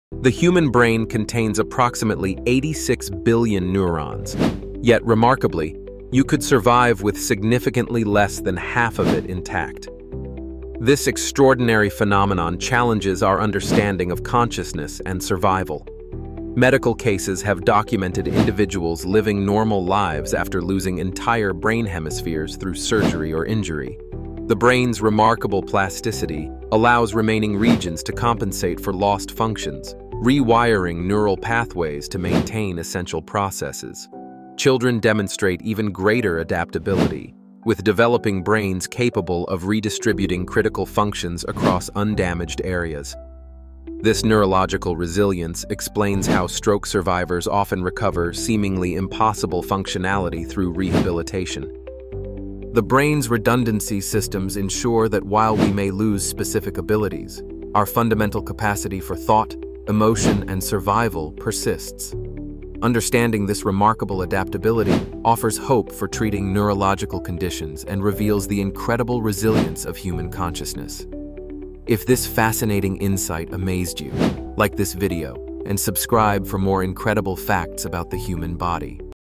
Did You Know You Can Survive Without a Large Part of Your Brain?
The human brain is one of the most remarkable organs in our body, containing an astounding 86 billion neurons working together to create consciousness, memories, and control our every function. What's even more incredible is that you could lose more than half of your brain and still live a completely normal life. This isn't science fiction – it's documented medical reality. Cases exist of people who have had entire brain hemispheres removed due to surgery or injury, yet they continue to think, feel, and function just like anyone else. The secret lies in the brain's extraordinary plasticity and ability to rewire itself. When one area is damaged, other regions step up to take over those responsibilities, creating new neural pathways and connections. This adaptability is especially pronounced in children, whose developing brains can redistribute critical functions across healthy areas with remarkable efficiency. It's the same principle that allows stroke survivors to regain abilities that doctors once thought were permanently lost through dedicated rehabilitation. Our brains have built-in redundancy systems that protect our core consciousness and survival instincts, even when faced with significant damage. This incredible resilience not only showcases the marvel of human biology but also provides hope for breakthrough treatments in neurological conditions.